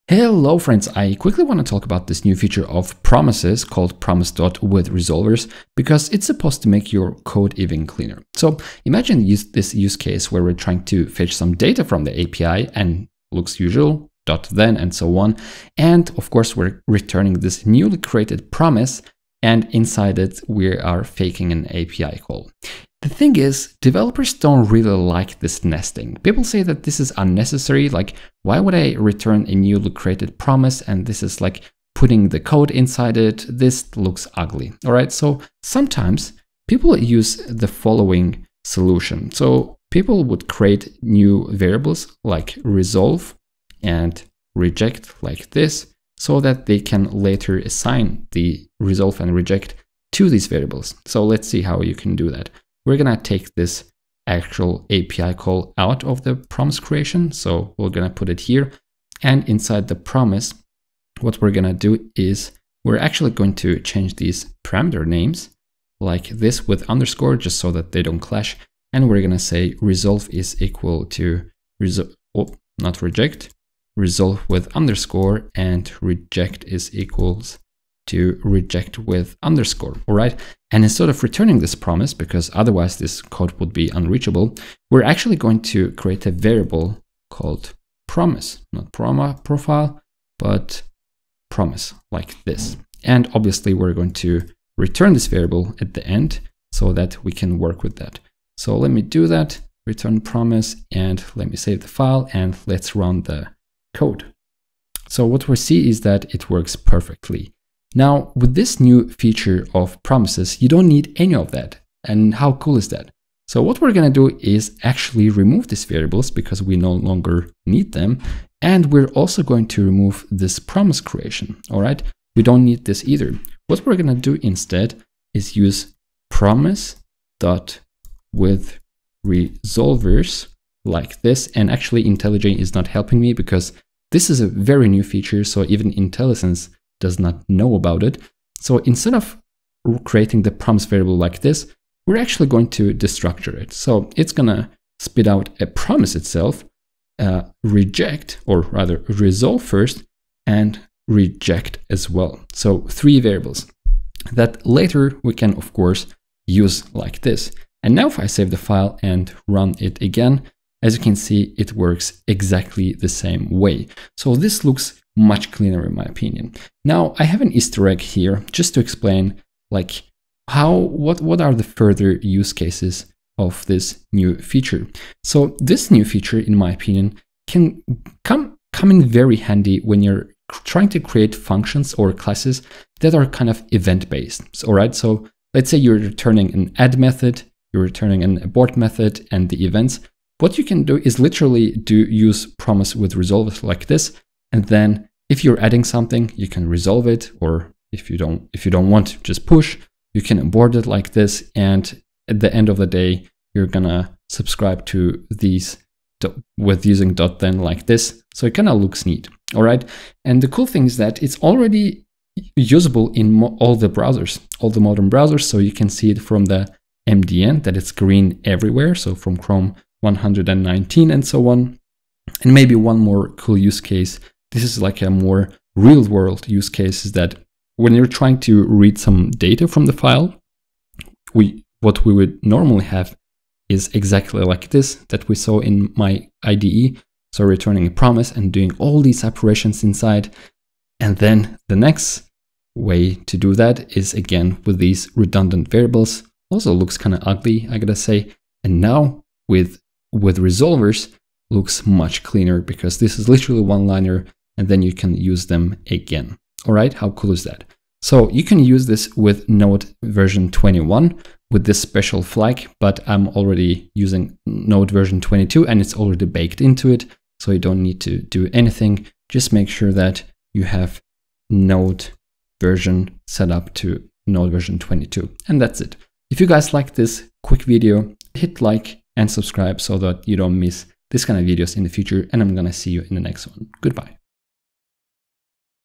Promises have just gotten nicer! (withResolvers)
Promise.withResolvers() lets you create Promises in a more elegant way just by calling one function.

Consider becoming a member of the channel by joining me ❤️